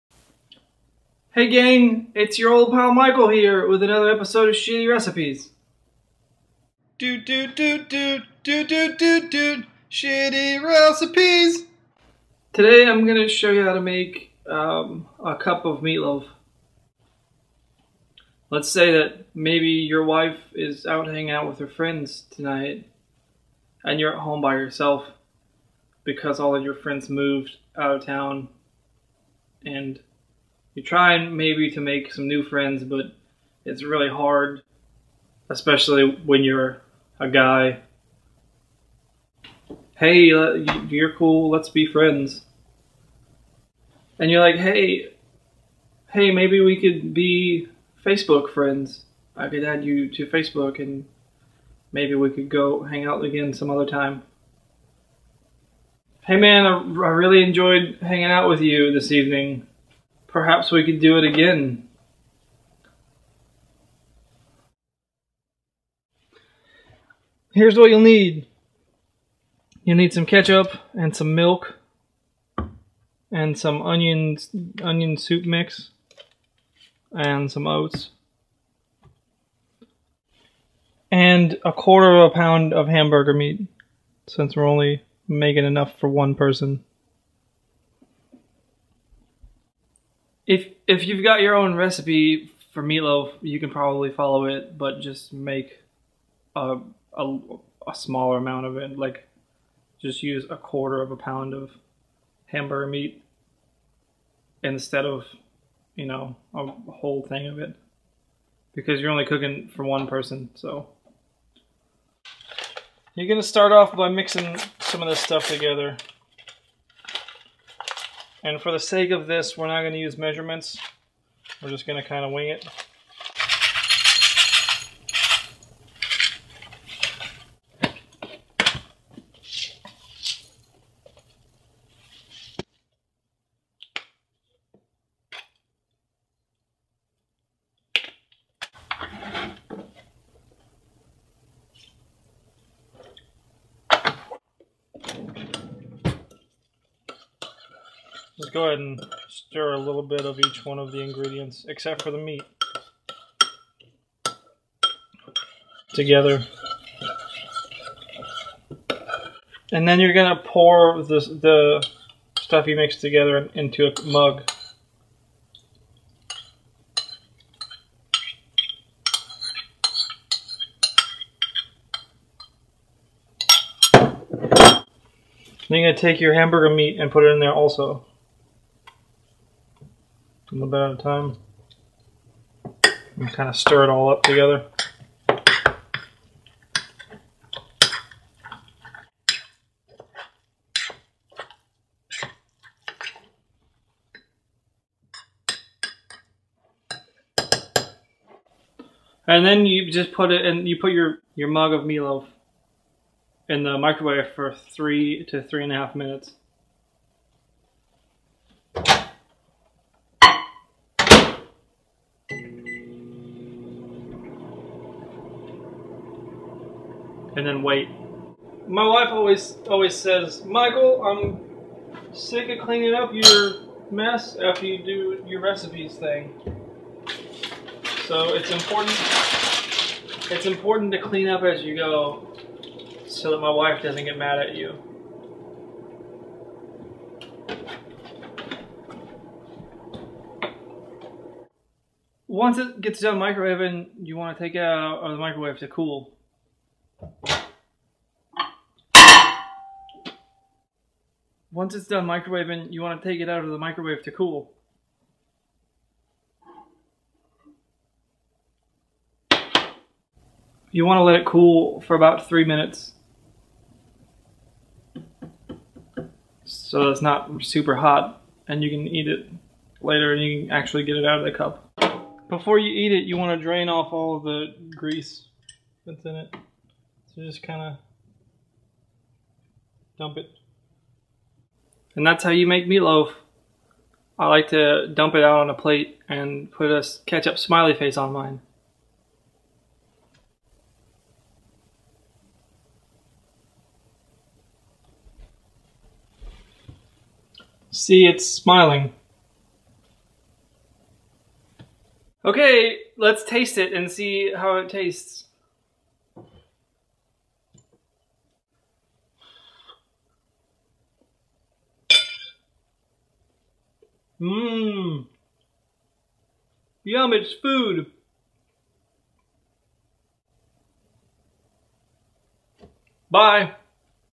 Meatloaf in a Cup - Shitty Recipes, Volume 9
Meatloaf is already pretty easy to make. It's just meat in the shape of a loaf. You could probably reshape your meatloaf and call it something other than meatloaf, like a meatstar or meatface. Meatballs follow the same principle. I'm going to show you how to make meatloaf in a cup in your microwave. It's super easy and also very delicious.